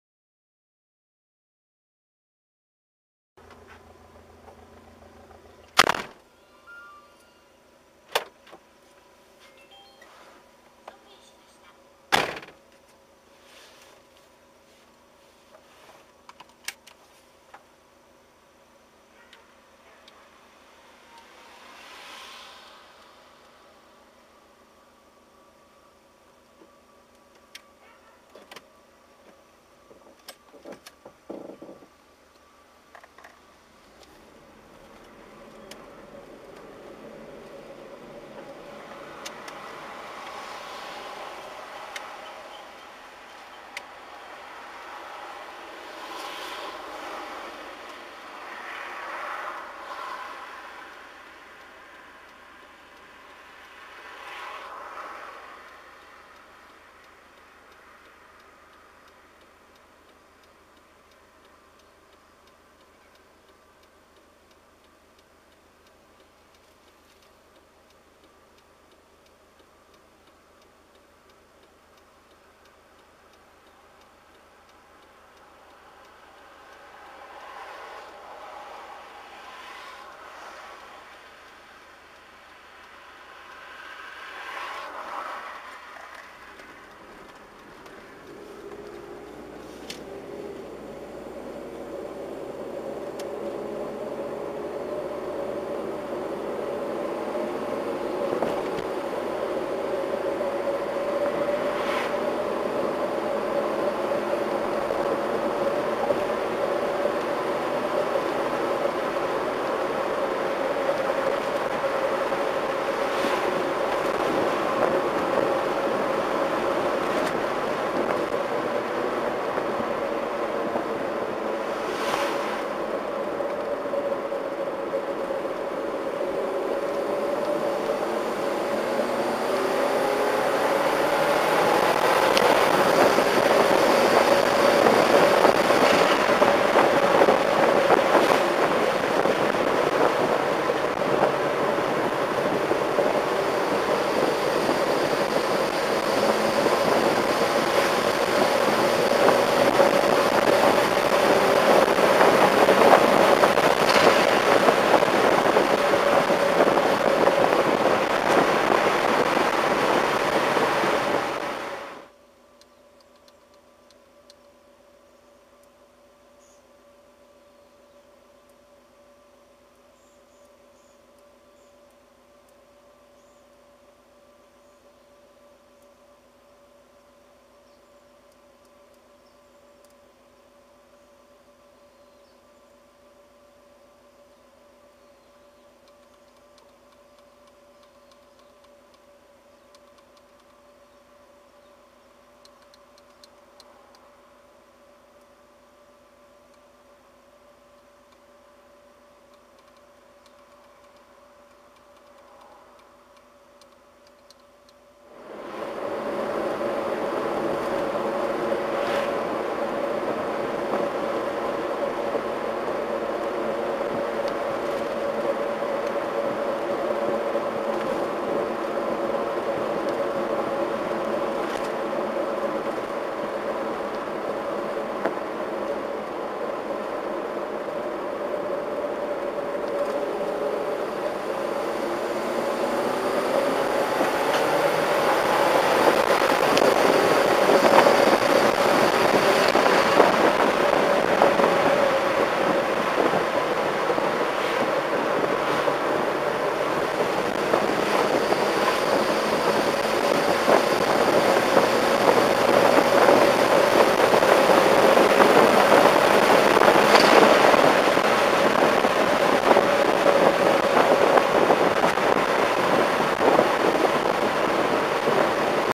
Real-time Rom Emulator Test
Tommykaira M20b Rom tunes by GRID Real-time Emulator